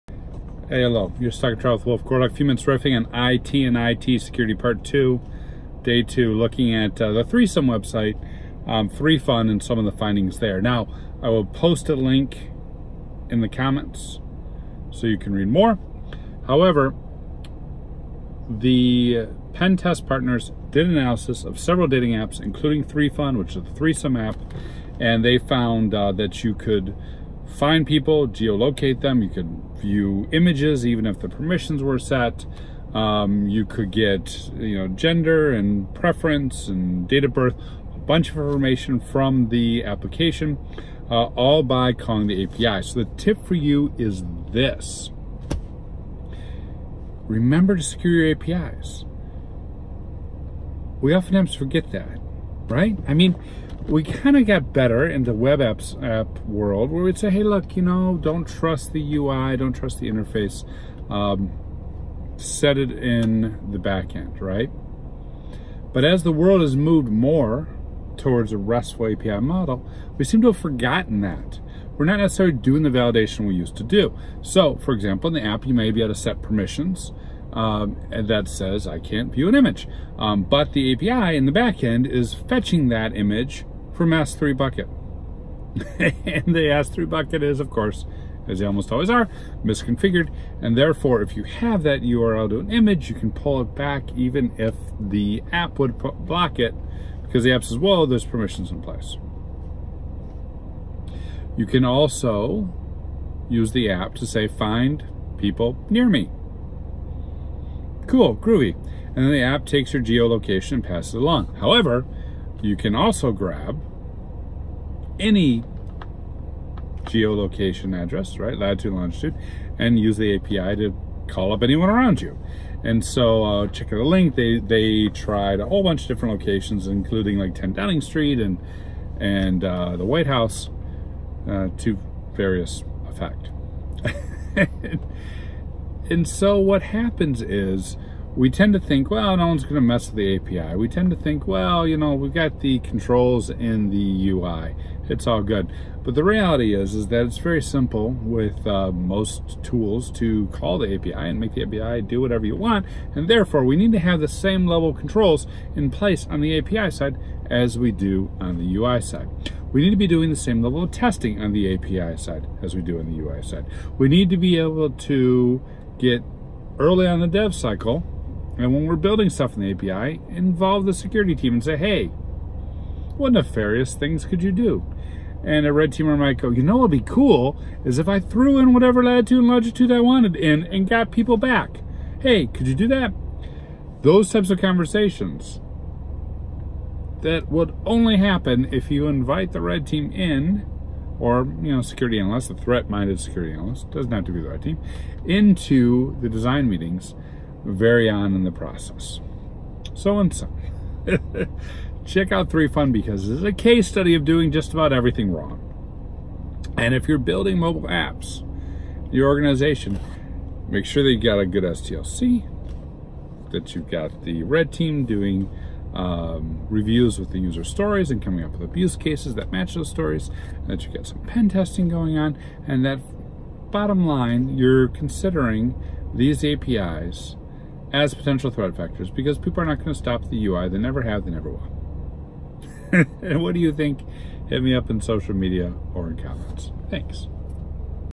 Stuck in Traffic - Three Ways to Protect Three-some APIs
Another look at 3fun’s mistakes, and three lessons we can take back to our mobile AppDev teams.